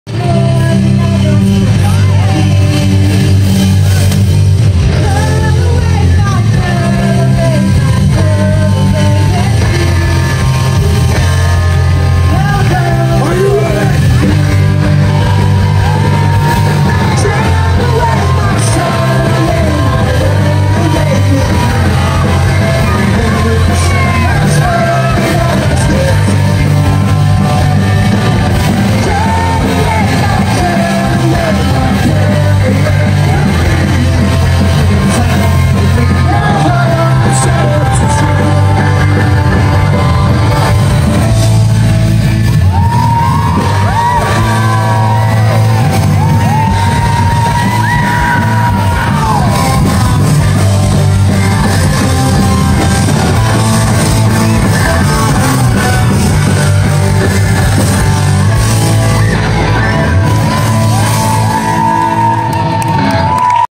Flatfoot 56 @ Audiofeed 2018
Vanessa & her little bun joining the boys.